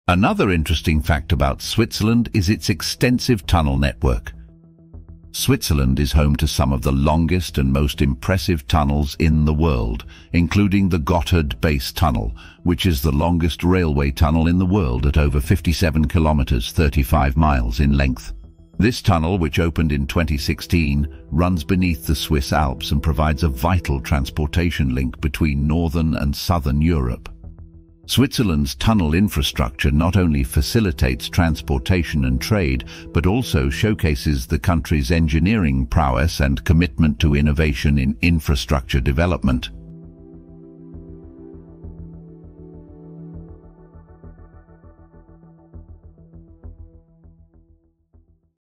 The World Longest Railway Tunnel?
Switzerland's extensive tunnel network is a testament to the country's engineering prowess, innovation, and commitment to facilitating efficient transportation and connectivity through challenging terrain.

GlobeGliders